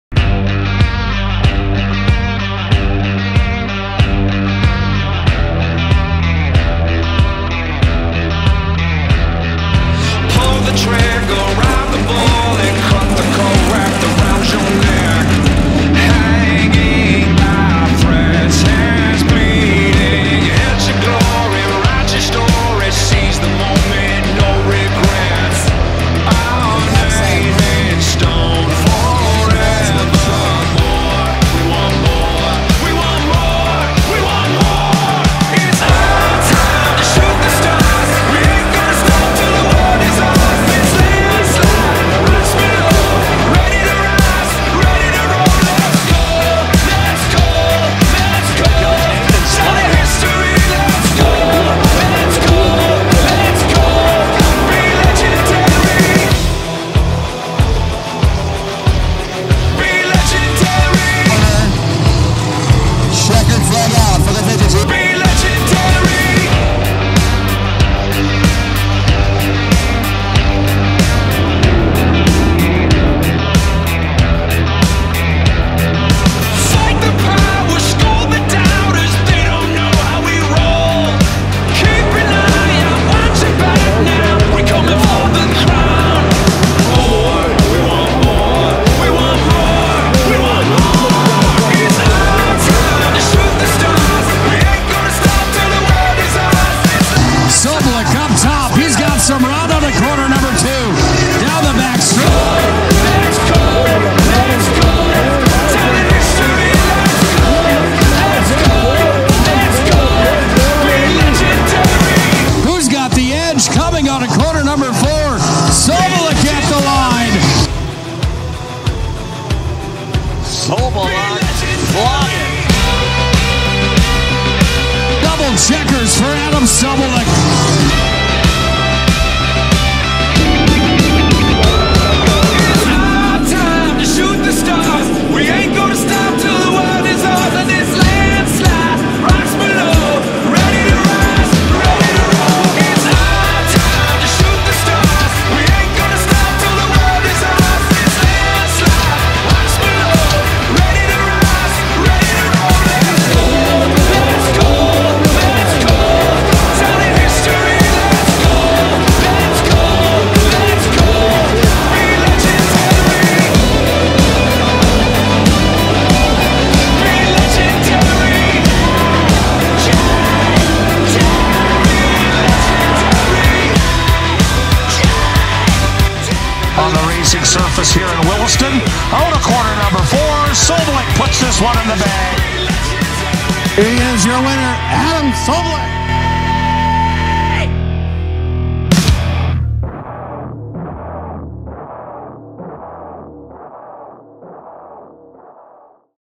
#52 Adam Sobolik 2021 Season Highlights
Adam drives the #52 Wingless Sprint out of Grand Forks, ND.